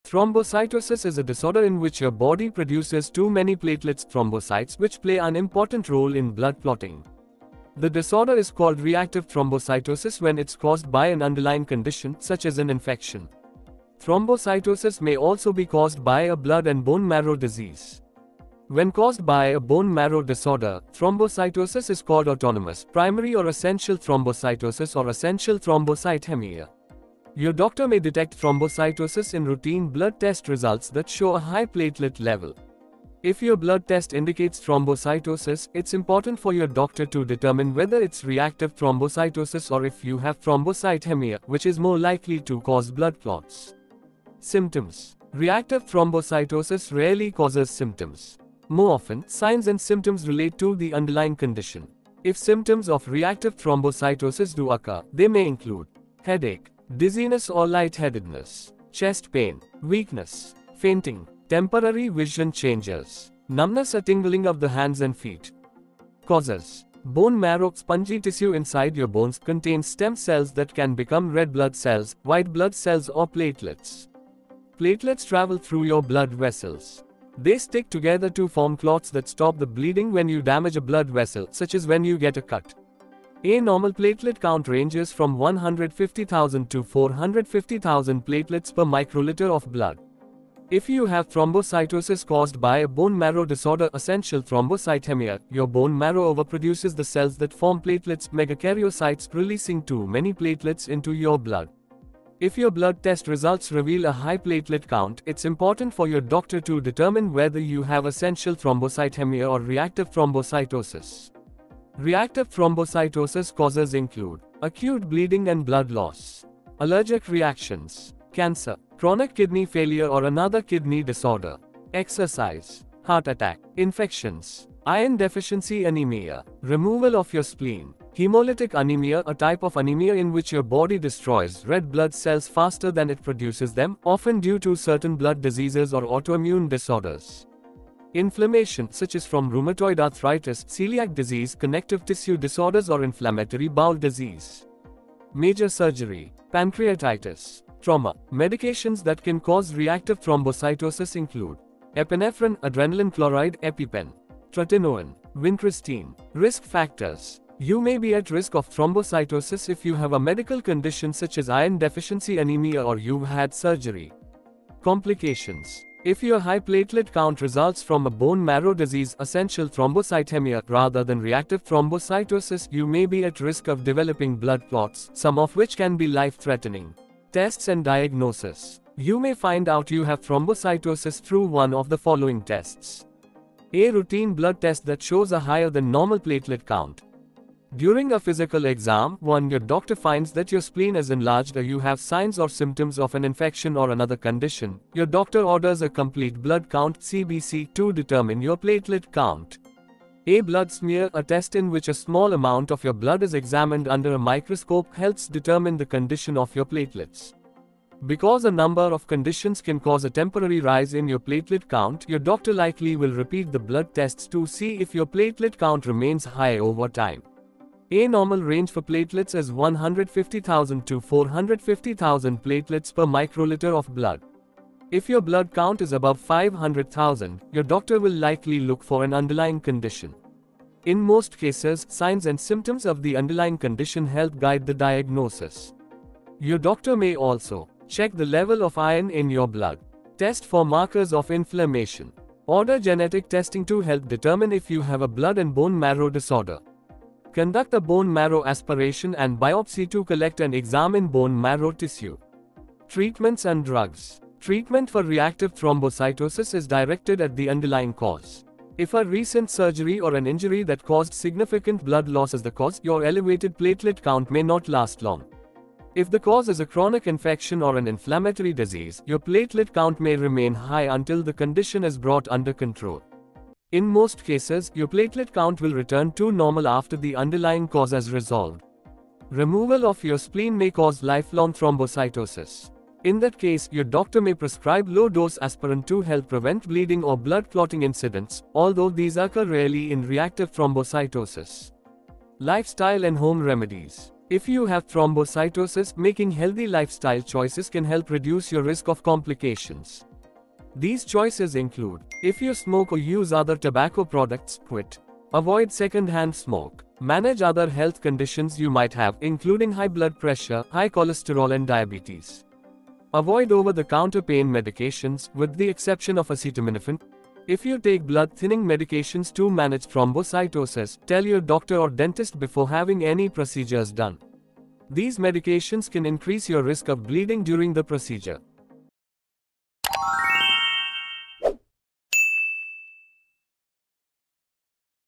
THROMBOCYTOSIS (OR) High Platelet Count - Cause - Symptoms - Complications - Treatment
Thrombocytosis  is a disorder in which your body produces too many platelets (thrombocytes), which play an important role in blood clotting. The disorder is called reactive thrombocytosis when it's caused by an underlying condition, such as an infection.

Thrombocytosis may also be caused by a blood and bone marrow disease. When caused by a bone marrow disorder, thrombocytosis is called autonomous, primary or essential thrombocytosis or Essential thrombocythemia.

Your doctor may detect thrombocytosis in routine blood test results that show a high platelet level. If your blood test indicates thrombocytosis, it's important for your doctor to determine whether it's reactive thrombocytosis or if you have thrombocythemia, which is more likely to cause blood clots.

SYMPTOMS :
Reactive thrombocytosis rarely causes symptoms. More often, signs and symptoms relate to the underlying condition. If symptoms of reactive thrombocytosis do occur, they may include:

. Headache
. Dizziness or lightheadedness
. Chest pain
. Weakness
. Fainting
. Temporary vision changes
. Numbness or tingling of the hands and feet

CAUSES :
Bone marrow — spongy tissue inside your bones — contains stem cells that can become red blood cells, white blood cells or platelets. Platelets travel through your blood vessels. They stick together to form clots that stop the bleeding when you damage a blood vessel, such as when you get a cut. A normal platelet count ranges from 150,000 to 450,000 platelets per microliter of blood.

If you have thrombocytosis caused by a bone marrow disorder (Essential thrombocythemia), your bone marrow overproduces the cells that form platelets (megakaryocytes), releasing too many platelets into your blood. If your blood test results reveal a high platelet count, it's important for your doctor to determine whether you have Essential thrombocythemia or reactive thrombocytosis.

Reactive thrombocytosis causes include:

. Acute bleeding and blood loss
. Allergic reactions
. Cancer
. Chronic kidney failure or another kidney disorder
. Exercise
. Heart attack
. Infections
. Iron deficiency anemia
. Removal of your spleen
. Hemolytic Anemia — a type of Anemia in which your body destroys red blood cells faster than it produces them, often due to certain blood diseases or autoimmune disorders
. Inflammation, such as from Rheumatoid arthritis, Celiac disease, connective tissue disorders or inflammatory bowel disease
. Major surgery
. Pancreatitis
. Trauma
. Medications that can cause reactive thrombocytosis include:

. Epinephrine (Adrenalin Chloride, EpiPen)
. Tretinoin
. Vincristine

RISK FACTORS:
You may be at risk of thrombocytosis if you have a medical condition such as Iron deficiency anemia or you've had surgery.

COMPLICATIONS :
If your high platelet count results from a bone marrow disease (Essential thrombocythemia), rather than reactive thrombocytosis, you may be at risk of developing blood clots, some of which can be life-threatening.

TESTS AND DIAGNOSIS :
You may find out you have thrombocytosis through one of the following tests:

A routine blood test that shows a higher than normal platelet count.
During a physical exam, when your doctor finds that your spleen is enlarged or you have signs or symptoms of an infection or another condition, your doctor orders a complete blood count (CBC) to determine your platelet count.
A blood smear — a test in which a small amount of your blood is examined under a microscope — helps determine the condition of your platelets.

TREATMENTS AND DRUGS :
Treatment for reactive thrombocytosis is directed at the underlying cause. If a recent surgery or an injury that caused significant blood loss is the cause, your elevated platelet count may not last long. If the cause is a chronic infection or an inflammatory disease, your platelet count may remain high until the condition is brought under control. In most cases, your platelet count will return to normal after the underlying cause is resolved.

Removal of your spleen may cause lifelong thrombocytosis. In that case, your doctor may prescribe low-dose aspirin to help prevent bleeding or blood-clotting incidents, although these occur rarely in reactive thrombocytosis.